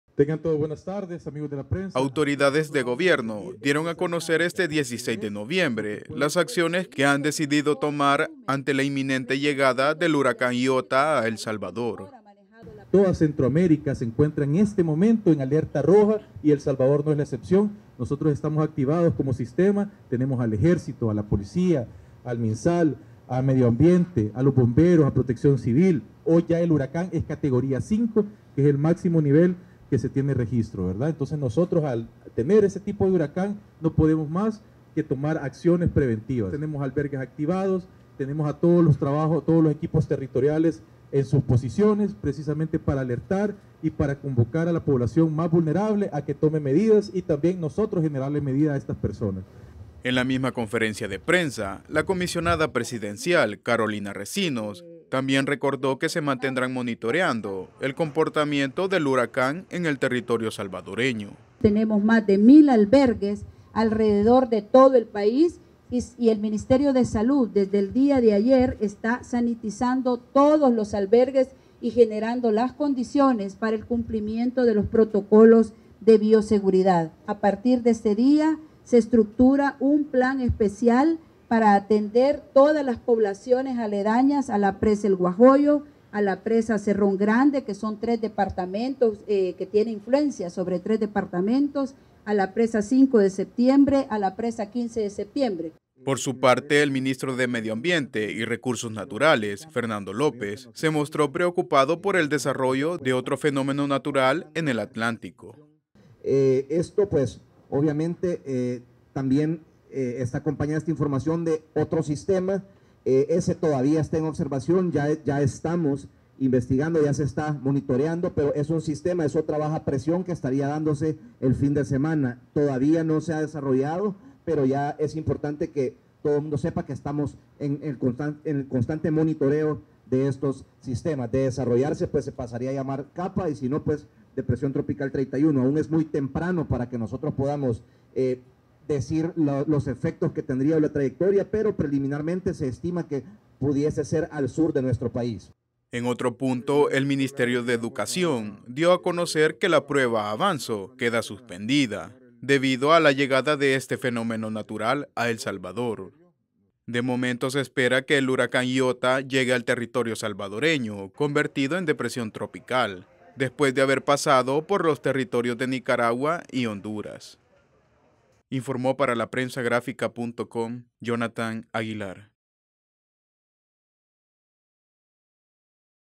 Llegada de Iota a El Salvador activa medidas de prevención
En otro punto, el Ministerio de Educación dio a conocer que la prueba AVANZO queda suspendida, debido a la llegada de este fenómeno natural a El Salvador.

¡Visítenos!